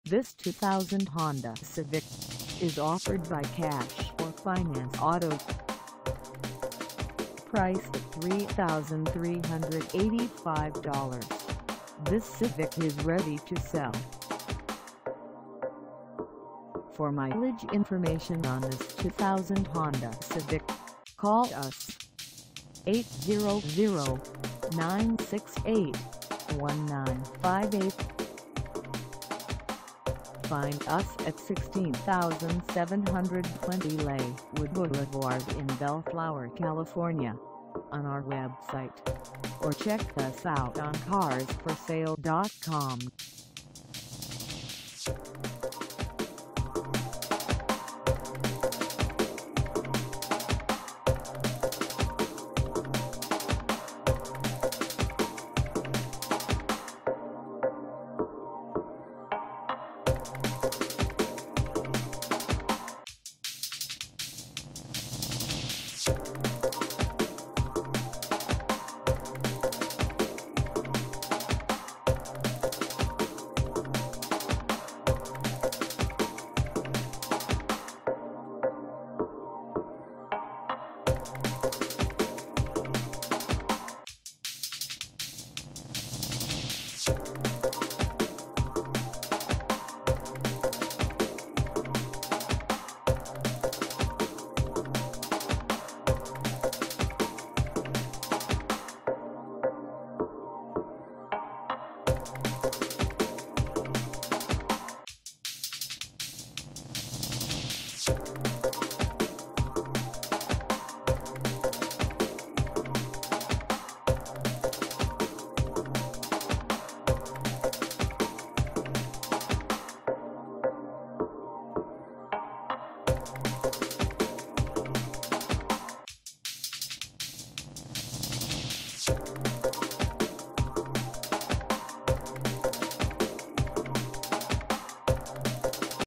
2000 Honda Civic EX 4dr Sedan for sale in BELLFLOWER, CA 907
This 2000 Honda Civic EX 4dr Sedan is for sale in BELLFLOWER, CA 90706 at Cash or Finance Auto.
Engine: I4 1.6L I4
Transmission:
Color: Gold
Mileage: 0
Contact Cash or Finance Auto 16720 Lakewood Blvd BELLFLOWER, CA 90706 to test drive this 2000 Honda Civic EX 4dr Sedan today.